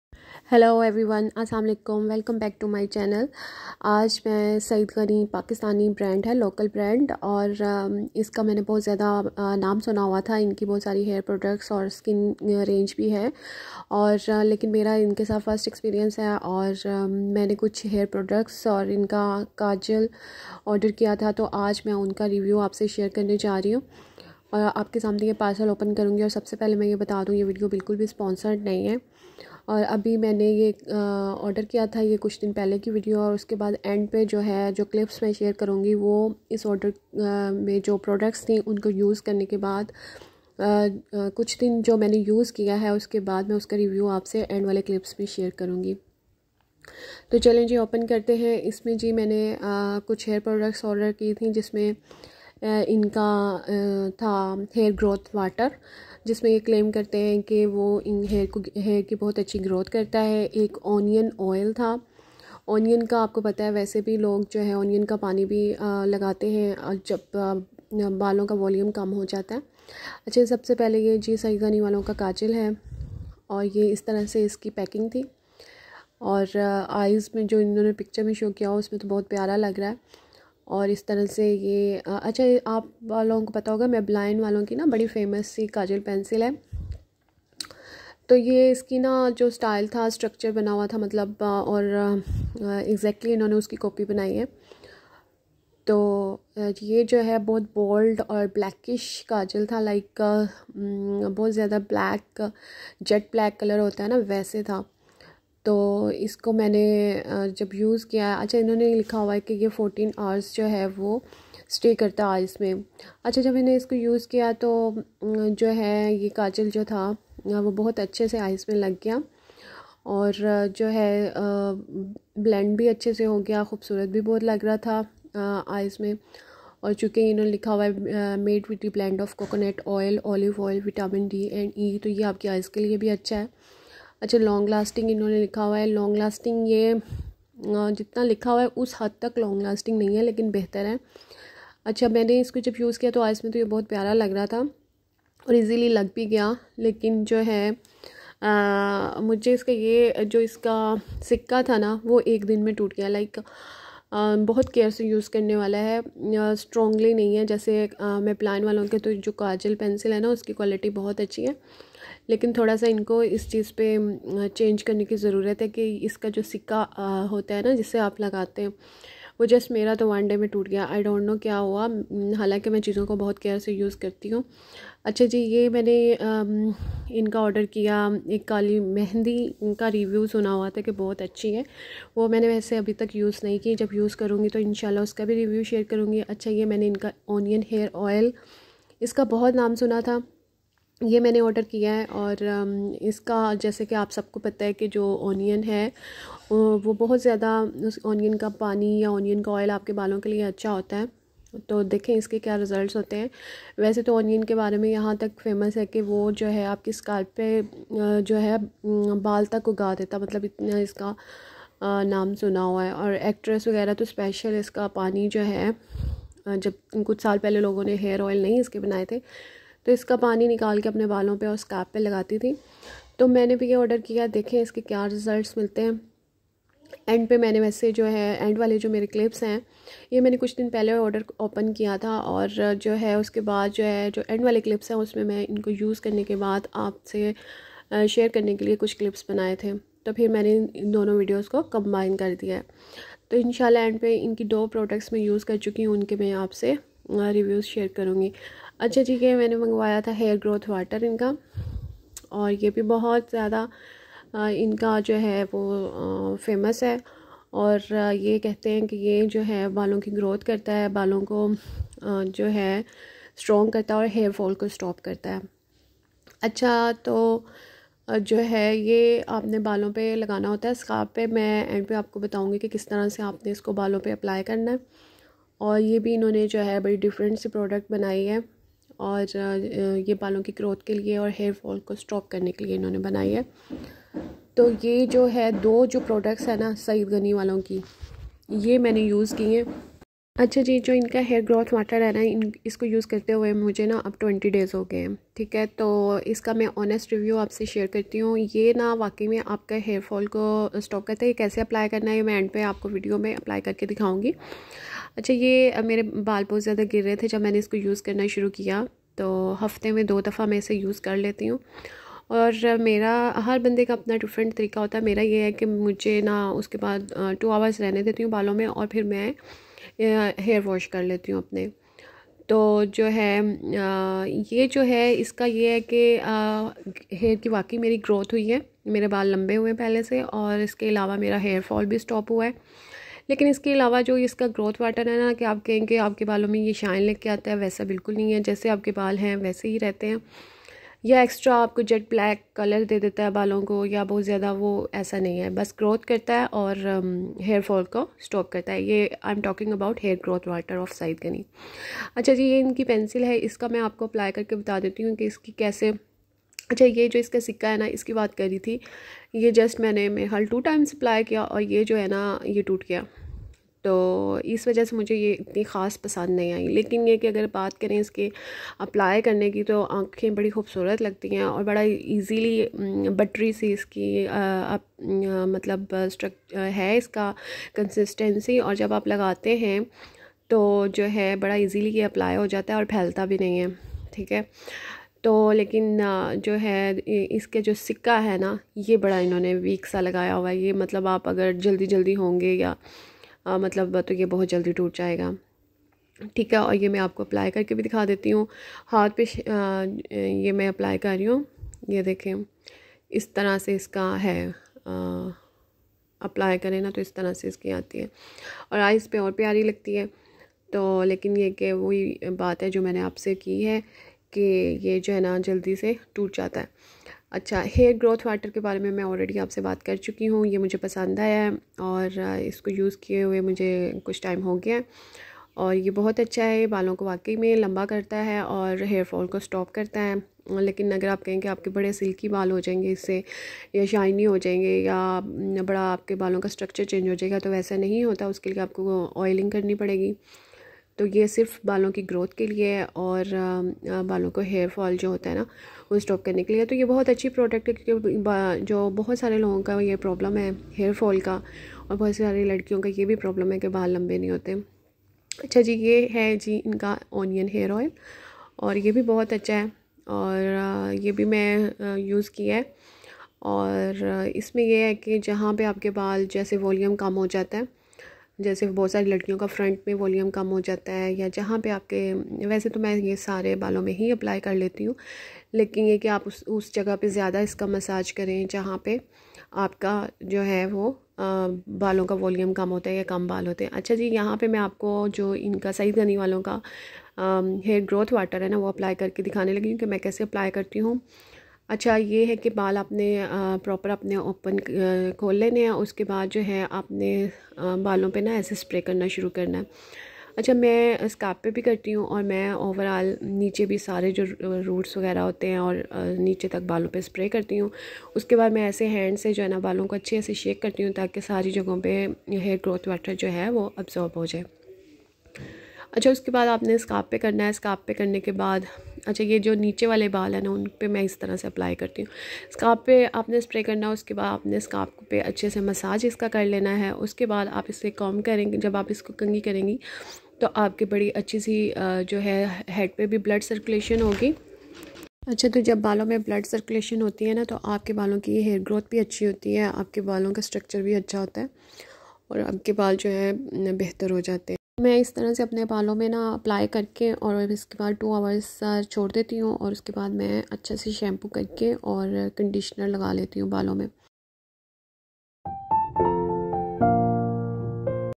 My hair care routine/Saeed ghani hair products honest review
in this video I will share my experience with saeed ghani products and their review